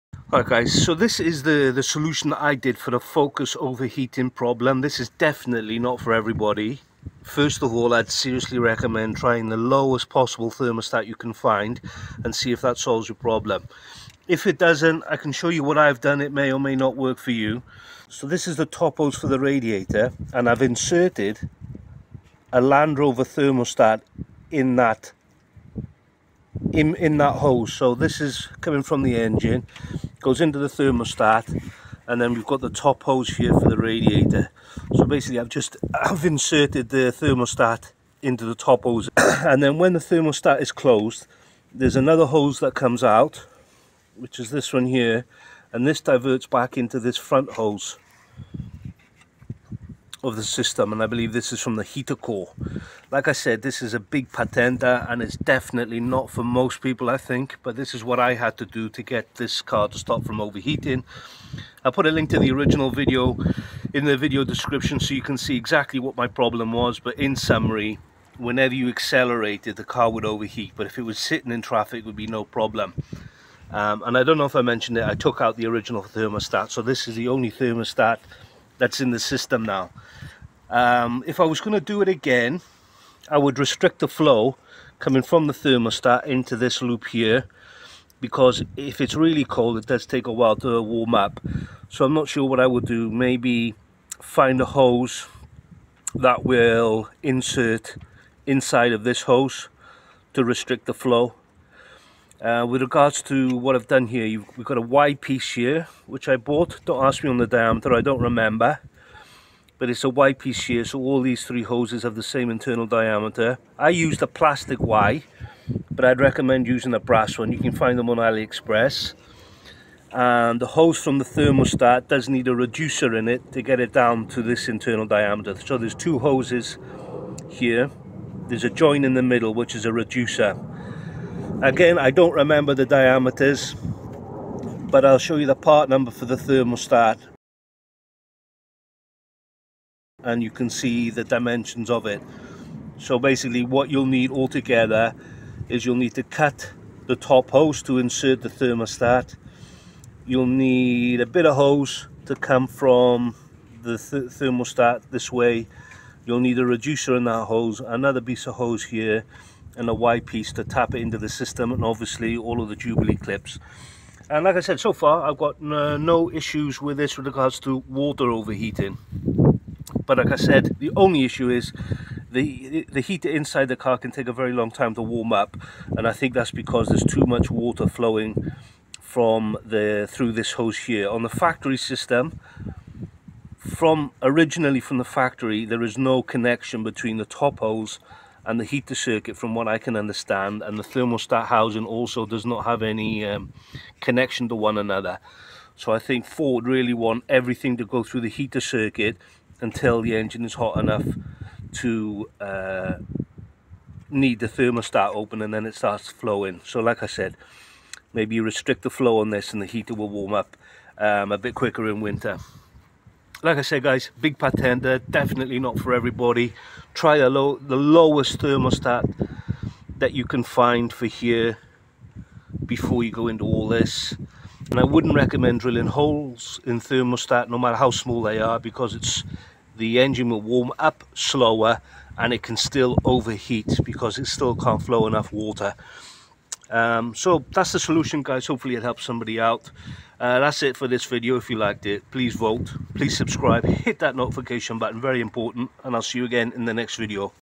Ford Focus Overheating Problem - FIX
How I fixed the Ford Focus overheating problem. Car would overheat under load but be fine sitting in traffic. Removing the thermostat stopped the overheating. This fix installs a Land Rover external thermostat inline with the radiator top hose. And when the thermostat is closed the water is routed to the heater circuit. And the stock thermostat is removed. I've had no water overheating issues since. Reducing the flow to the heater core hoses may help the heater come up to temp faster when temps are less than 7ish C.

Time Stamps (Click the time to go to that part of the video)

0:00 Solutions
1:02 The problem I had
1:21 Improvement
2:20 External thermostat
3:42 Don't do this